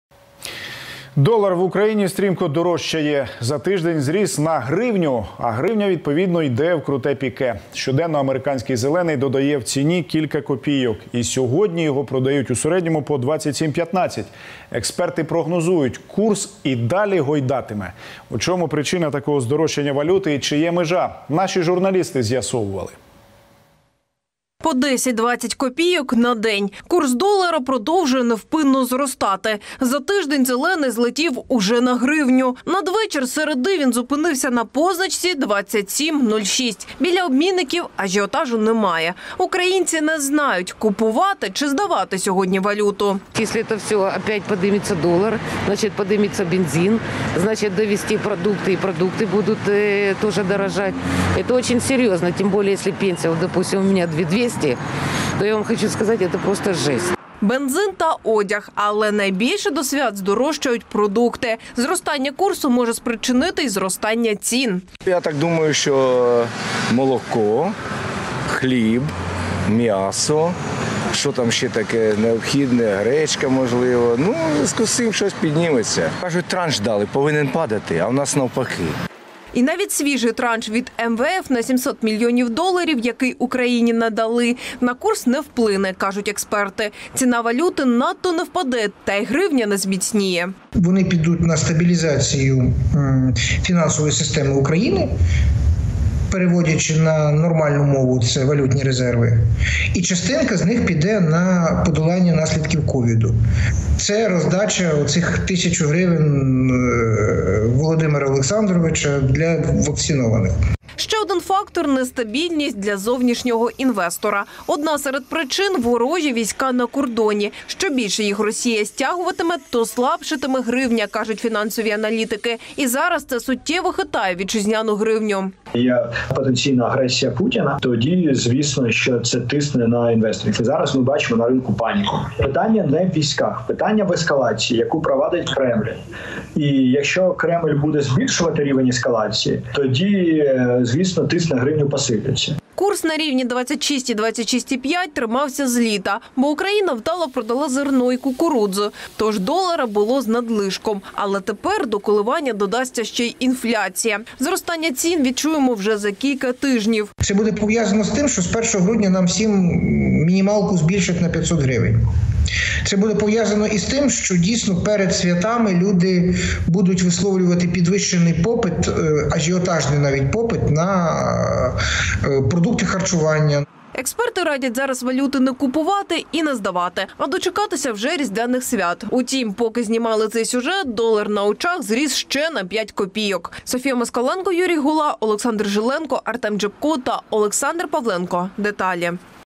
В Україні дорожчає долар: причини
Долар в Україні стрімко дорожчає. За тиждень зріс – на гривню! А гривня, відповідно, йде в круте піке. Щоденно "американський зелений" додає в ціні кілька копійок. І сьогодні його продають уже по 27,06. У чому причина такого здорожчання валюти і чи є межа? З’ясовували журналісти "Деталей".